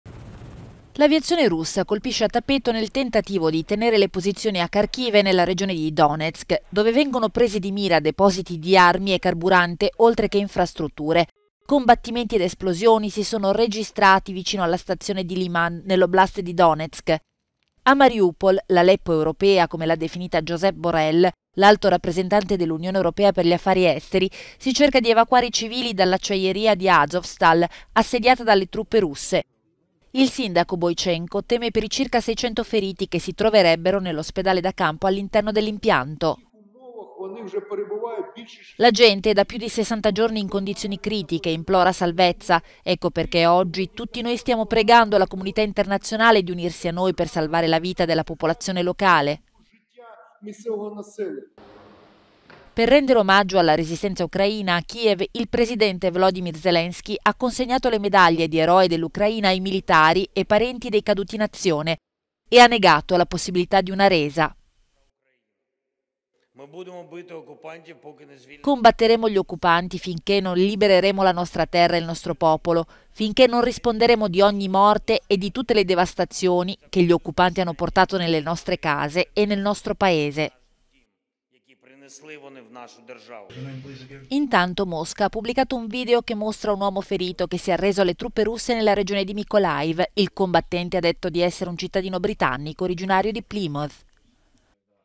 Mariupol, l'appello del sindaco: "Aiutateci a salvare la vita della popolazione locale"
Non si fermano i bombardamenti russi in Ucraina. Il sindaco di Mariupol lancia un appello alla comunità internazionale